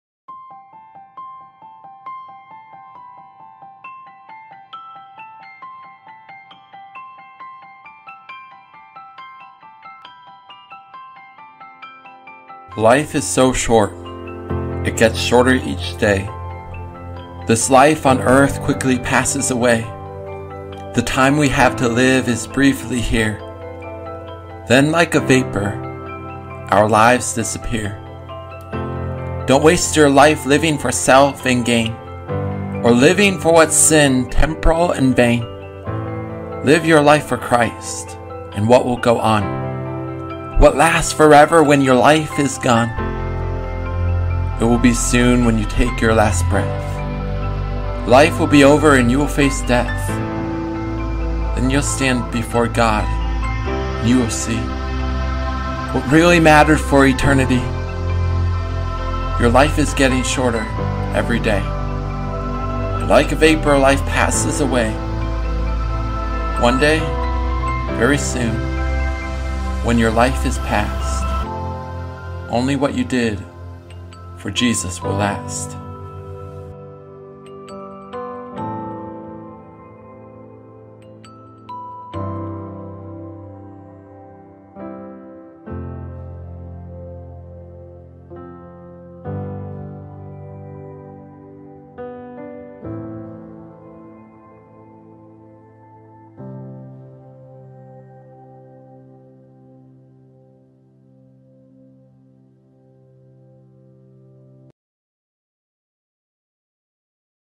LIFE by Joe Henkel | Advance 3D Animation
This is a college assignment and the task is to create a kinetic animation using After effects and use some 3D elements to represent the ways of the meanings in the poem.